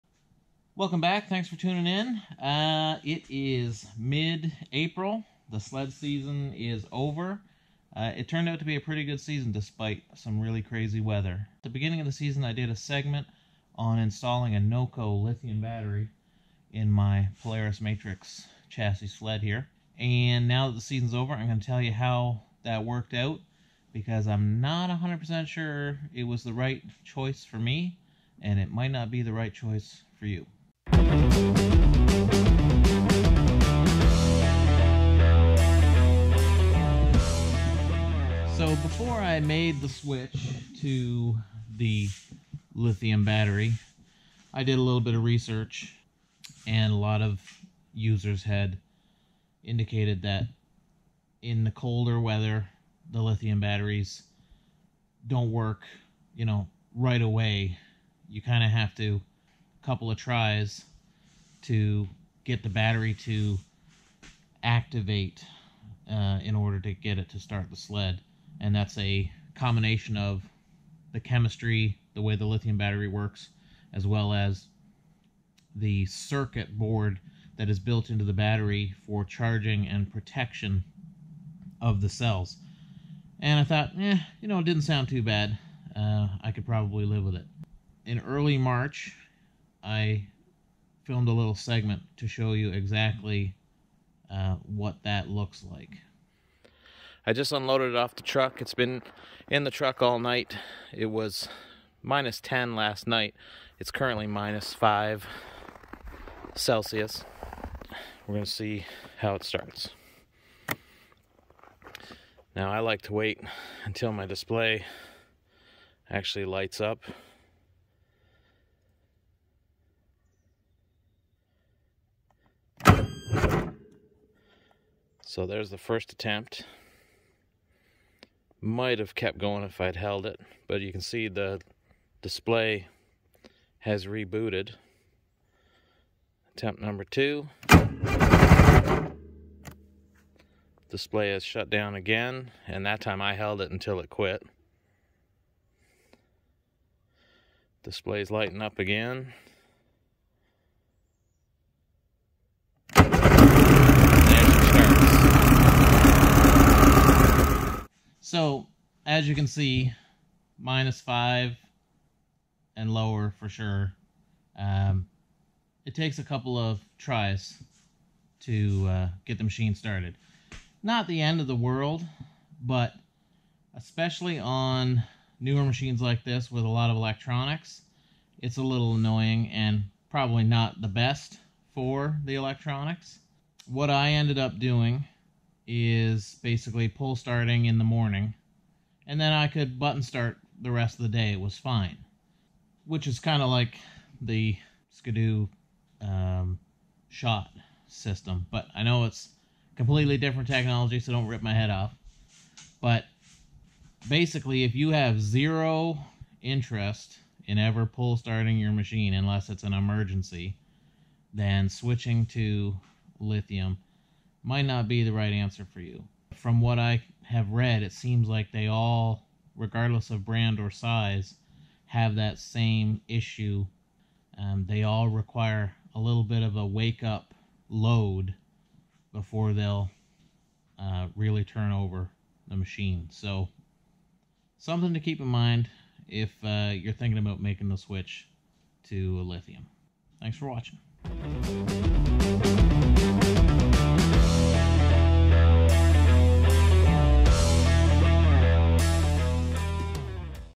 Lithium Battery In Snowmobile. The COLD Reality.
Is a lithium battery the right choice for your sled? Noco Lithium battery cold starting real world test. Polaris 850 VR1.